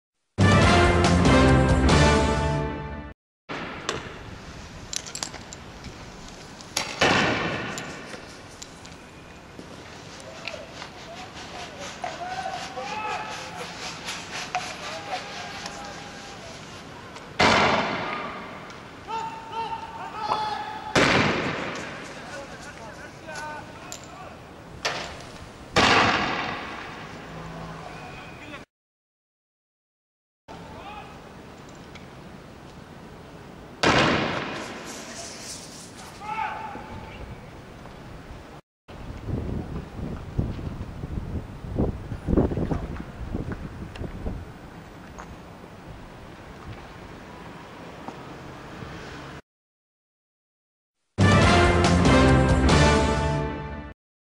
قوات النظام تقمع متظاهرين في النويدرات بالغازات السامة 2014\2\6 Bahrain
قمعت قوات النظام الخليفي مساء اليوم الخميس (6 فبراير/شباط 2014) متظاهرين في منطقة النويدرات جنوب العاصمة المنامة التي أدت الى مواجهات ضاربة بين متظاهرين وقوات النظام الحاكم بعد أن توجهوا نحو الشارع العام  تحت شعار "أعلناها منذ الآن .. موعدنا يوم العصيان" أستعداداً للاستحقاق الشعبي القادم تزامنا مع الذكرى الثالثة للثورة , حيث قابلتهم قوات النظام بالغازات السامة والرصاص الإشطاري الشوزن موقعة العديدي من الإصابات في صفوف المتظاهرين